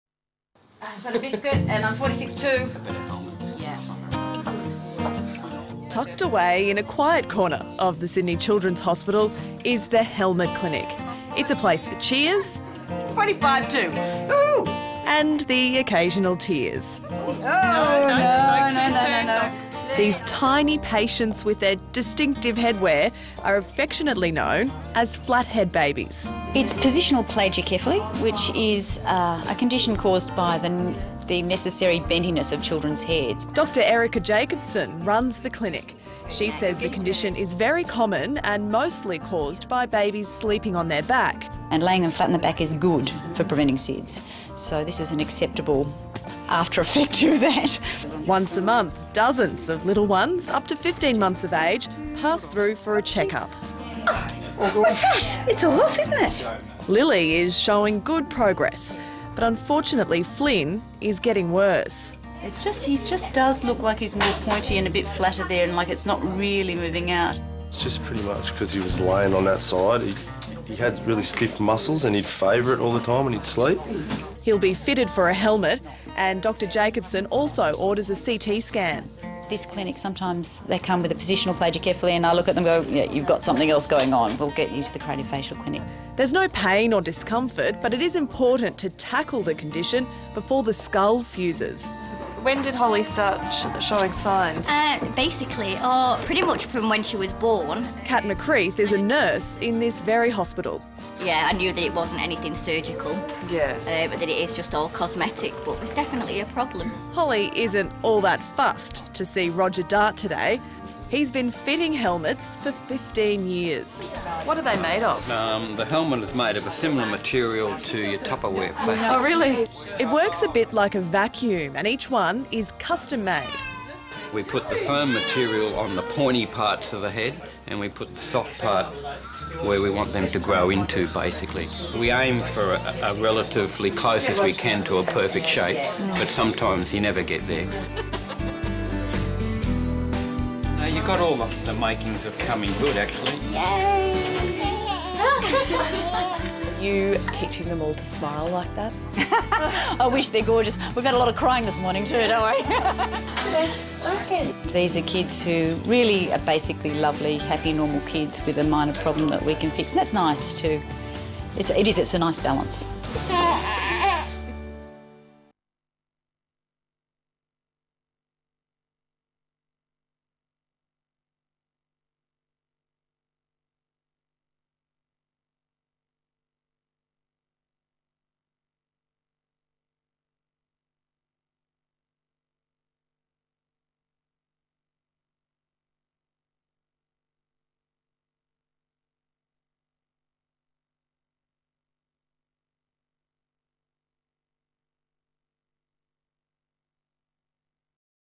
'Flat head' babies - Sydney Children's Hospital, Randwick
You might not have heard of 'flat head' babies but the doctors at Sydney Children's Hospital, Randwick treat their little patients with this common condition regularly.

For more information about the Hospital and Sydney Children's Hospital Foundation